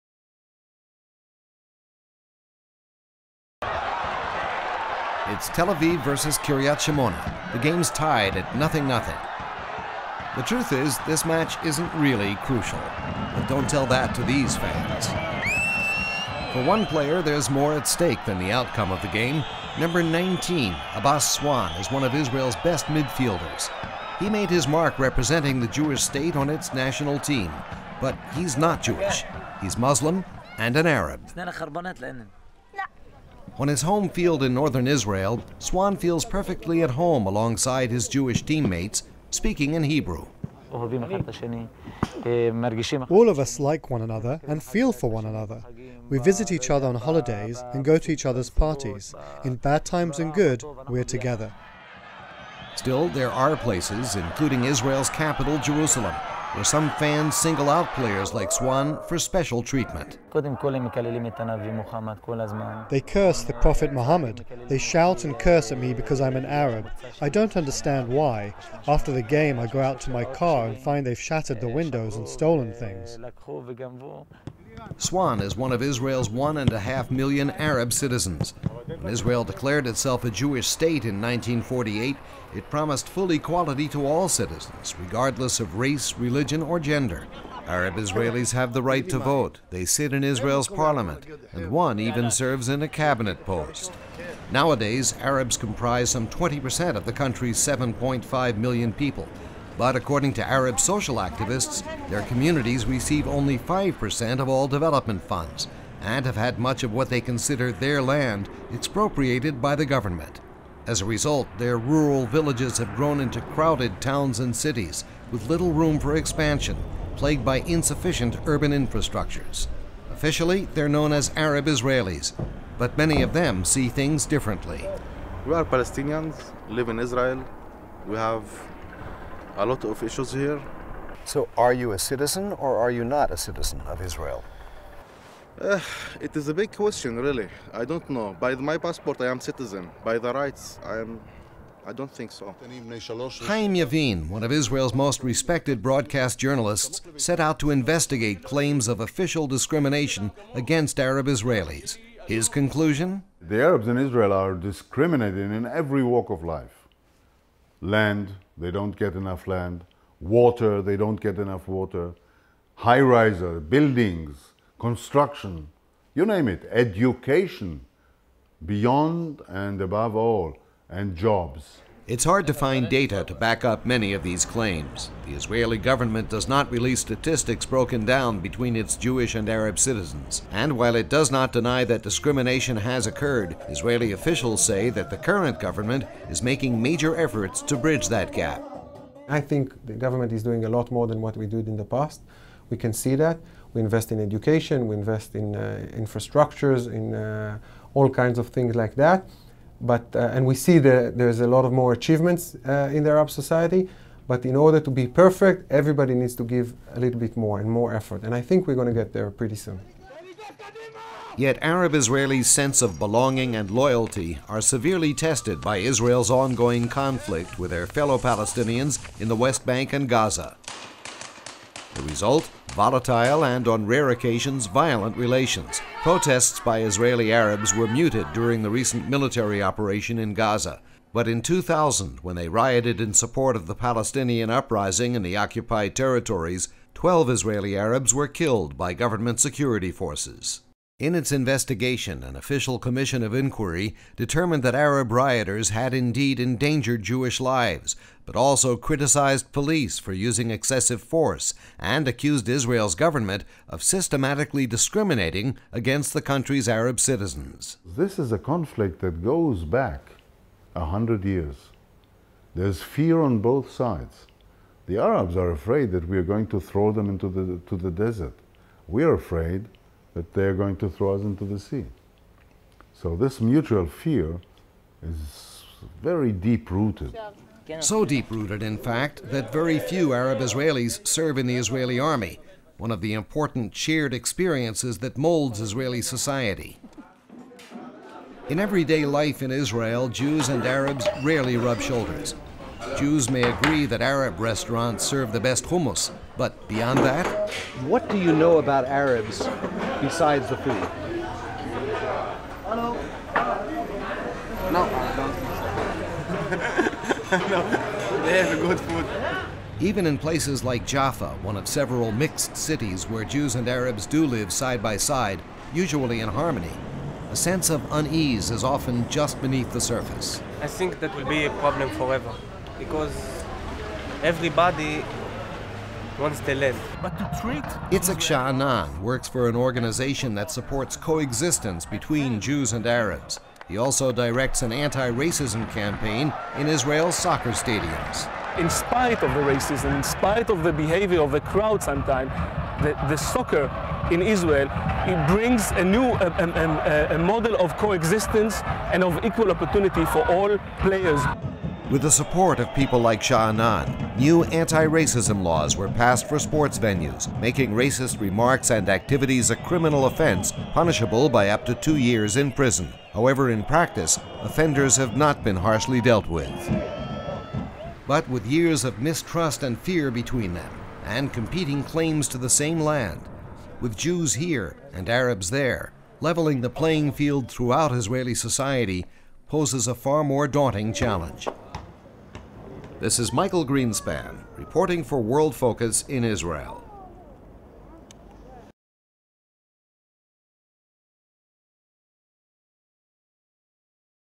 One in five Israeli citizens is Arab
The history of conflict between Israel and its Arab neighbors is well-documented. A story told not nearly as often is the relationship between Arabs and Jews within Israel, as approximately one in five Israelis is Arab.